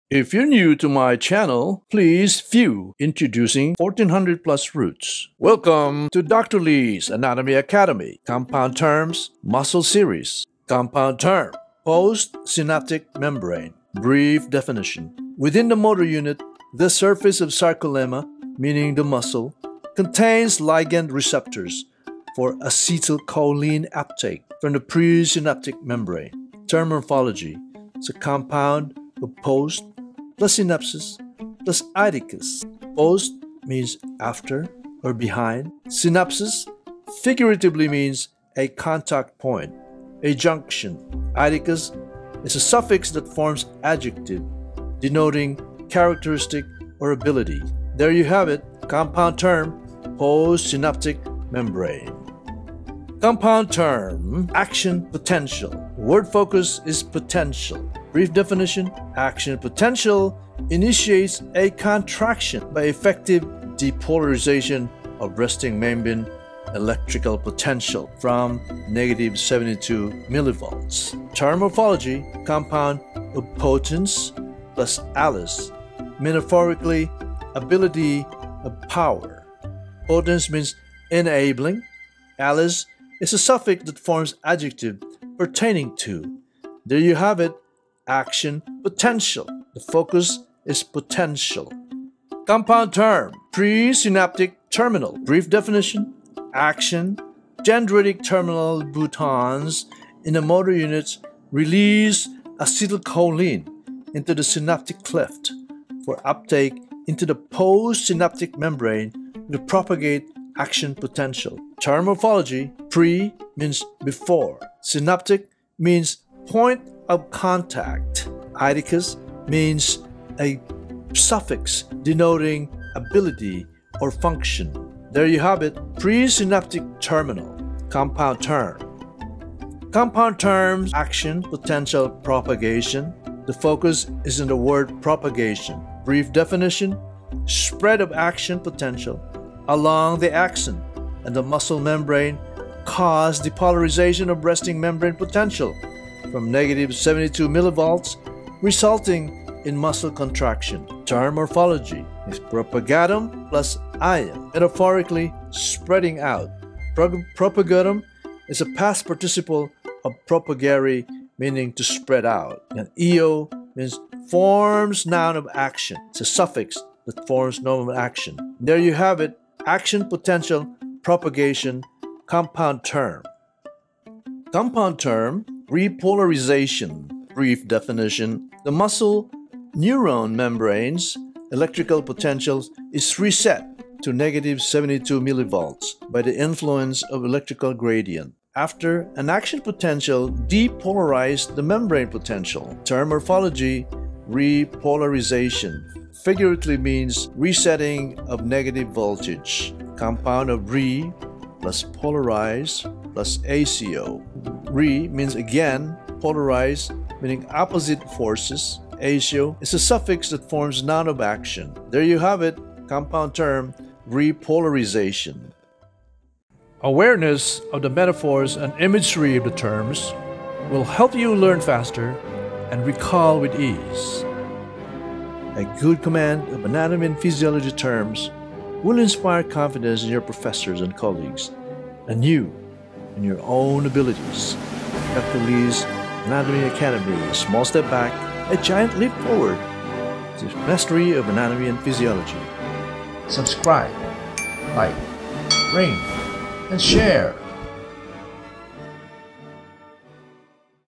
Compound Terms Series: Muscular System | EPISODE 14
The classical meaning of the root words is used for imagery of Anatomy and Physiology terms. Most terms have been Anglicized due to decline of Romance and Classical education; the figurative intent of the roots has been "Lost In Translation" or, simply, forgotten.

One root word may be used in one or a dozen terms across several body systems.

At. Dr. Lee Anatomy Academy, you will rediscover the linguistic foundation of Anatomy terms.

Chapters:
00:00 Intro
00:10 Post-Synaptic
00:53 Potential
01:32 Pre-Synaptic
02:09 Propagation
02:54 Depolarization
03:36 Outro

References:

Cassels New Latin Dictionary: Latin-English, English-Latin (1968) / edited by D. P. Simpson. New York: Funk & Wagnalls

Harper, Douglas ( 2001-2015), Retrieved from July/2014 – Dec./2014.

Oxford English Dictionary, The, Second Edition (1989) / edited by James A. H. Murray, Henry Bradley, W. A. Craigie and C. T. Onions. Oxford : Clarendon Press

Oxford Latin Dictionary / edited by P.G.W. Glare (1982). New York, NY: Oxford University Press

Random House Webster’s Unabridged Dictionary, Second Edition (2001). New York, NY: Random House, Inc.

Seeley, Rod R., Trent D. Stephens, Philip Tate (2007), Anatomy & Physiology, Sixth Edition. New York, NY: McGraw-Hill Higher education.

Cover: Latin Quote from Vesalius’ Anatomy Notes; retrieved Aug.